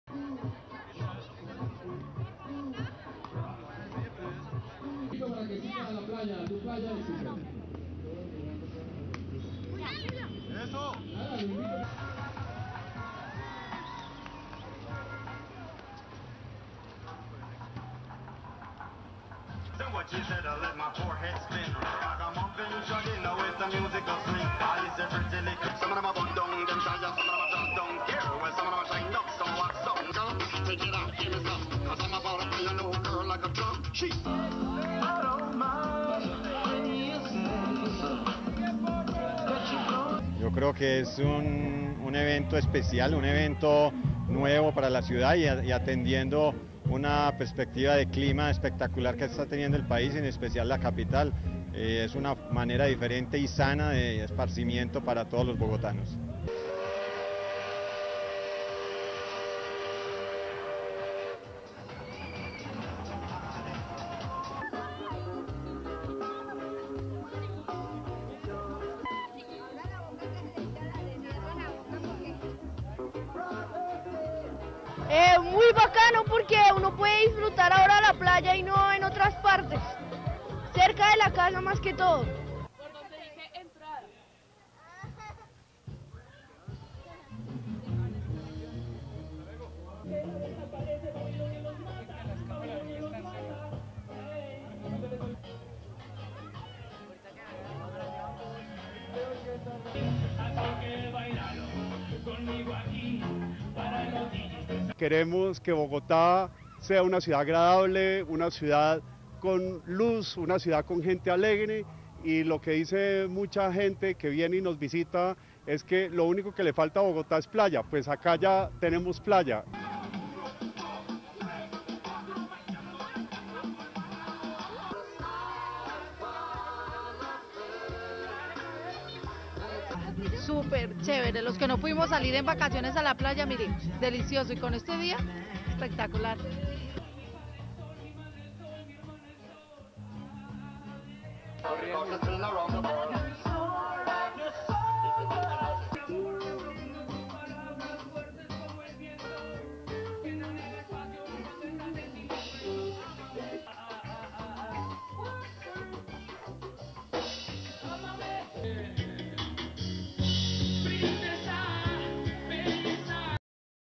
Authorities build artificial beaches around capital
1. Wide of buildings and people playing volleyball on artificial beach
2. Mid shot of women playing volleyball in swim suits
3. Exterior of Exito (one of the biggest stores in Colombia) and vehicles passing by while people play volleyball on the artificial beach
4. Women playing volleyball
5. Various of women in swimming suits sunbathing on artificial beach
6. People on the artificial beach
7. SOUNDBITE (Spanish) Alejandro Calle Arcila, Exito's vice president of marketing:
"I think this is a special event. A new event for the city and people are enjoying the spectacular weather conditions the country has, especially the capital. It's a different and healthy way of relaxing for all the residents of Bogota."
8. Train passing near Exito store, people on artificial beach
9. Mid shot of woman burying son with sand
10. Close of boy being buried with sand
11. SOUNDBITE (Spanish) Vox pop, Oscar Ramirez, Resident of Bogota:
"It's very nice because now we can enjoy the beach and it doesn't mean we have to go somewhere else to do so. It's nice because it is close to our house."
12. Various of children playing with sand on the artificial beach
13. Woman putting some sunblock cream on woman's back
14. Residents of Bogota on the artificial beach
15. SOUNDBITE (Spanish) Vox pop, Jose Luis Torres, Exito employee:
"We want Bogota to be a pleasant city. A sunny city. A city with happy people. What a lot of people who visit us say is that the only thing that Bogota needed is a beach and now we have it."
16. Various of women carrying fruit on their heads
17. Women sunbathing
18. SOUNDBITE (Spanish) Vox pop, Sandra Rivas, Resident of Bogota:
"I think this is very nice. Those who couldn't go out on vacation to the beach are enjoying this. This is very nice and with such a nice day it's spectacular."
19. Pan right of musicians playing reggae and people singing and dancing on the beach
20. People singing and dancing on the beach while musicians sing
21. Musicians playing reggae and beachgoers singing and dancing
22. Various of people playing volleyball on the beach
23. Life guard watching
STORYLINE
Bogota residents on Sunday enjoyed the pleasures brought by unseasonal good weather at a series of artificial beaches along the capital's main pedestrian street.
Several tonnes of sand were transported from warehouses in the city to make the five man-made beaches.
And Bogota is currently experiencing perfect beach weather.
The average temperature for this time of the year in the Colombian capital is 14.0�Celsius (57 Fahrenheit).
But during the last few days temperatures have reached 25.0�Celsius (77 Fahrenheit) and above.
"I think this is very nice. Those who couldn't go out on vacation to the beach are enjoining this. This is very nice and with such a nice day it's spectacular," said Sandra Rivas, a resident of Bogota who took her son for a day on the beach.
The beaches have been located in what is known in the capital as the "ciclovia".
Ciclovia is a Spanish term, meaning "cycling street," and is used in Latin America to mean either a permanent or temporary designated bicycle route.
Music, volleyball, games and, of course, people sunbathing, covered the white-sanded beaches.
"I think this is a special event. A new event for the city to enjoy the spectacular weather conditions the country has, specially in its capital," said Alejandro Calle Arcila, the vice president of marketing for a department chain store promoting the event.
"It's a different and healthy way of relaxing for all the residents of Bogota," said Arcila.